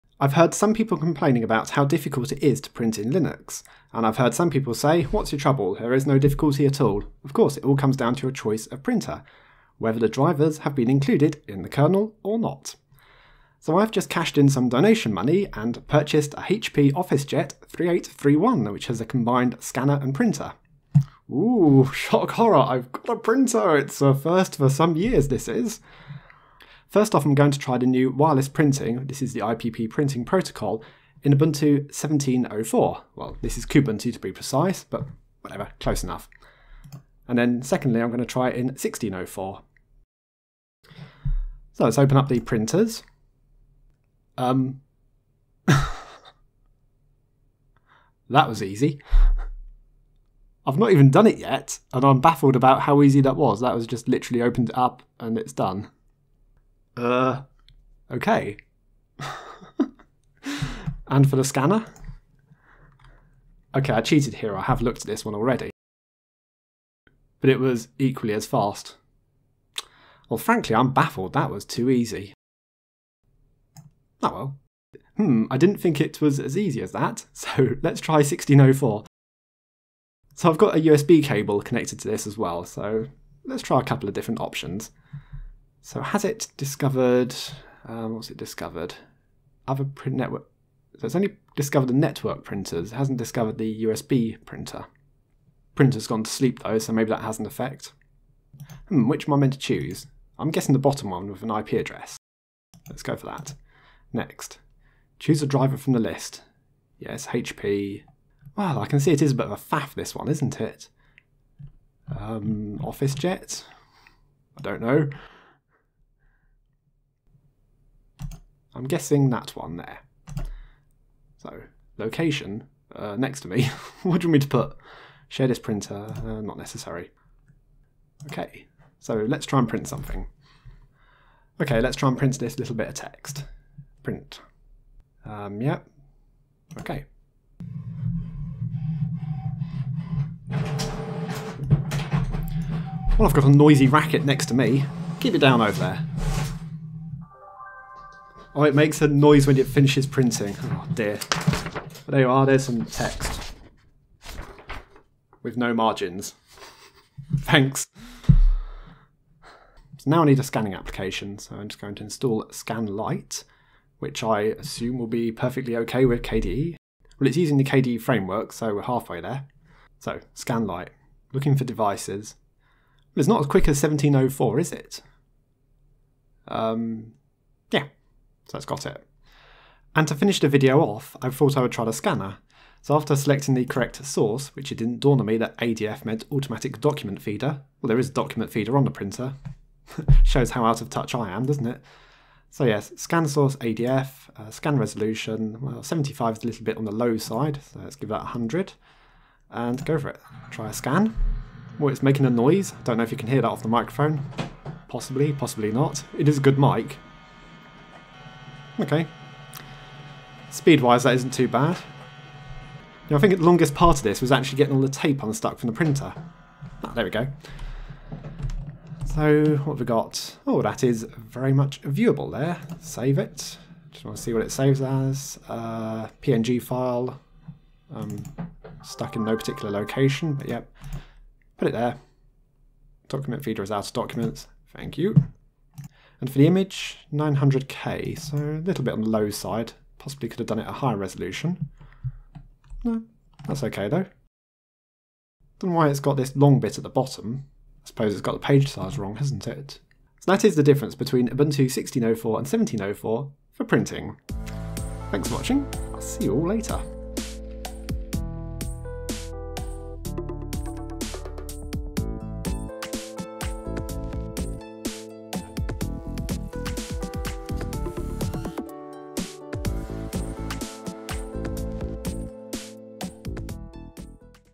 Driverless IPP Printing in Ubuntu 17.04 Compared to 16.04
I cashed in some donation money and purchased a HP Officejet 3831, which is  a combined Printer / Scanner. Ubuntu 17.04 with driverless IPP (Internet Printing Protocol) was vastly easier to get working compared to Ubuntu 16.04 (KDE Neon) where I had to choose a driver.
Overall it was still an easy experience to get a printer working in Linux.
Thanks to everyone who has donated to my channel.
Printer: HP Officejet 3831